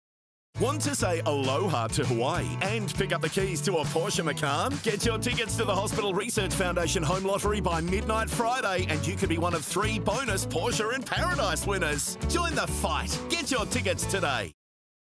HOME LOTTERY Bonus Draw
You could be 1 of 3 lucky winners driving away in a sleek Porsche Macan T AND flying away on a 7-night Hawaiian family escape. Get your Home Lottery tickets before midnight 9 June to be in the draw. Don’t miss out!